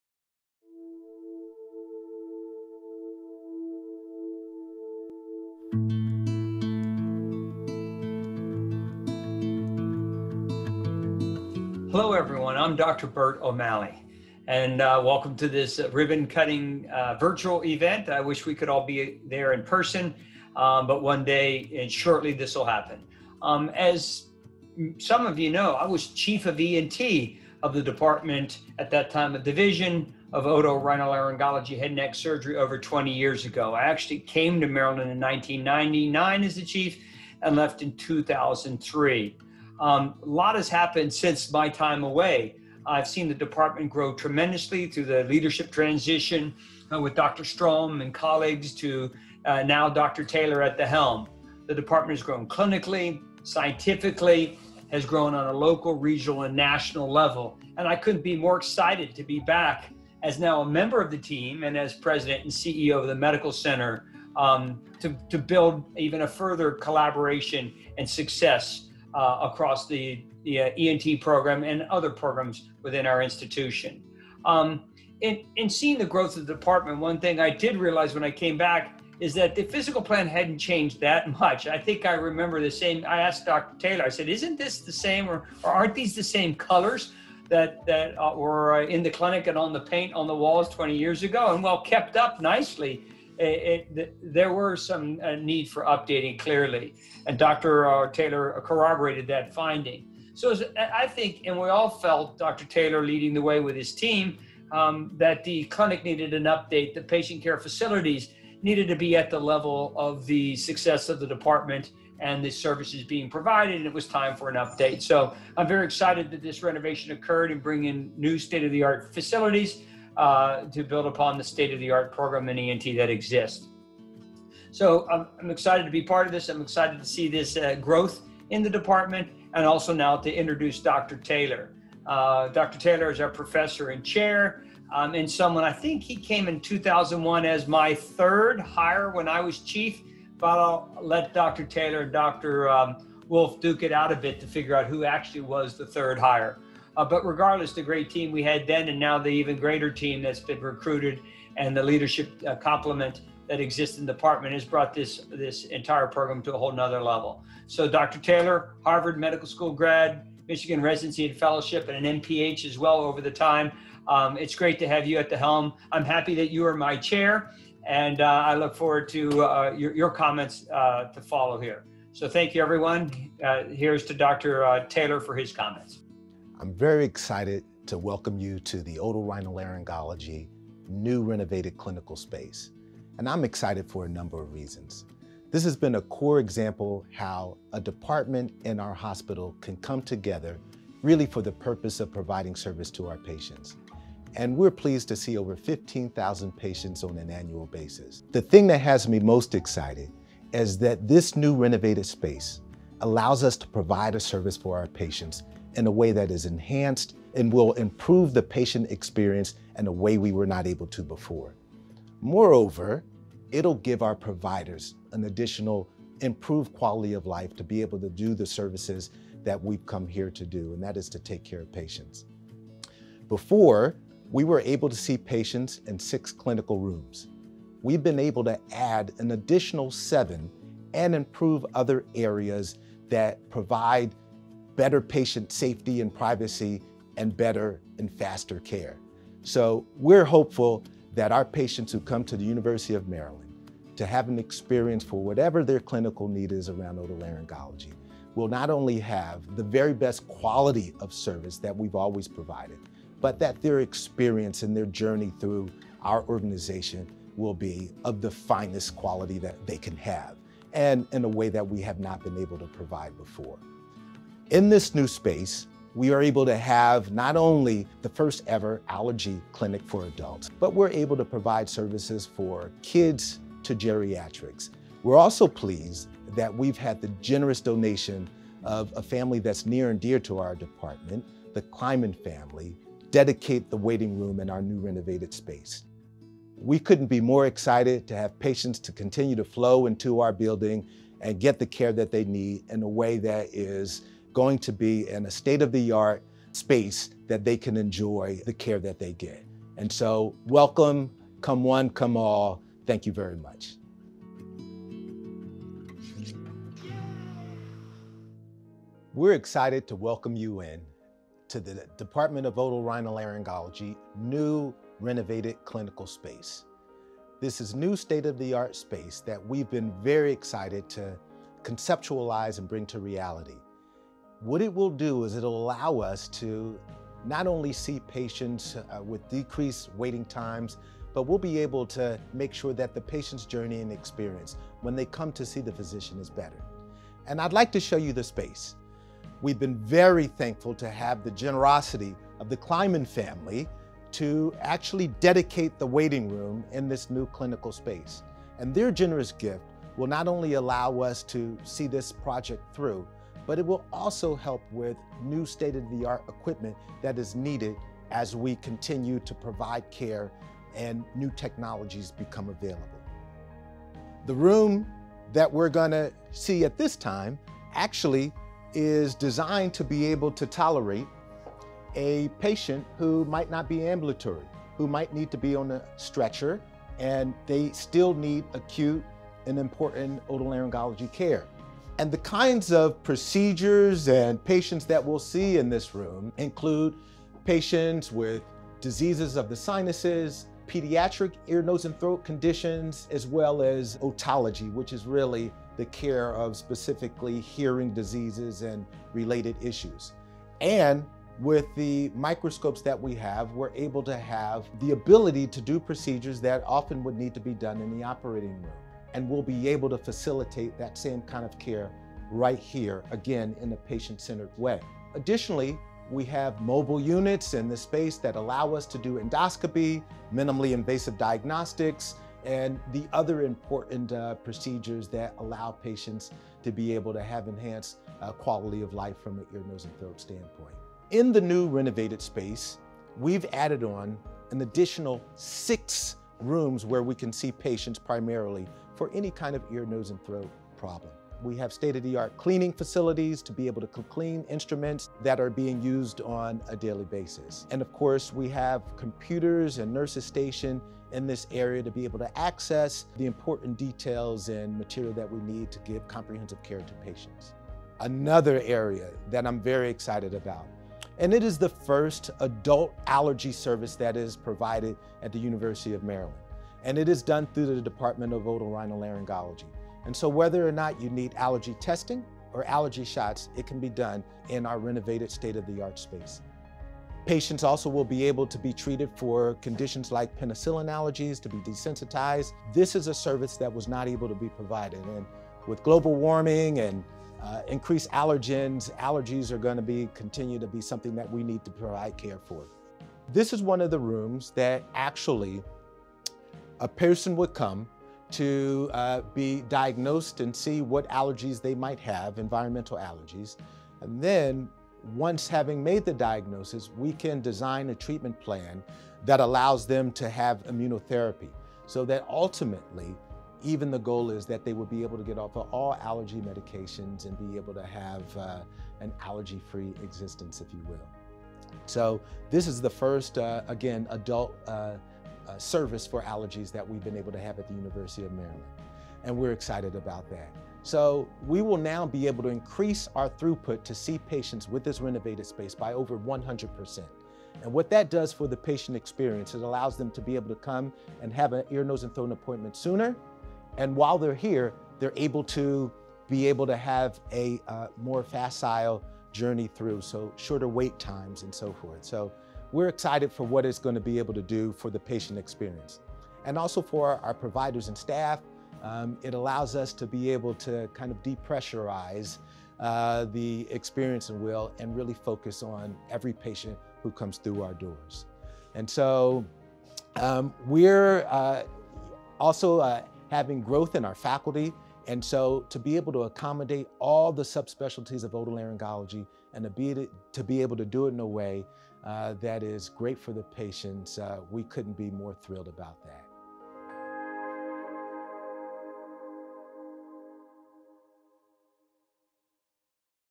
University of Maryland Medical Center New ENT Suite Virtual Ribbon Cutting
Watch as Drs. Bert O’Malley and Rodney Taylor welcome you into the expanded clinical ENT (Ear, Nose, and Throat) suite, which allows the expertise of UMMC’s nationally recognized head and neck clinicians to improve the lives of even more patients.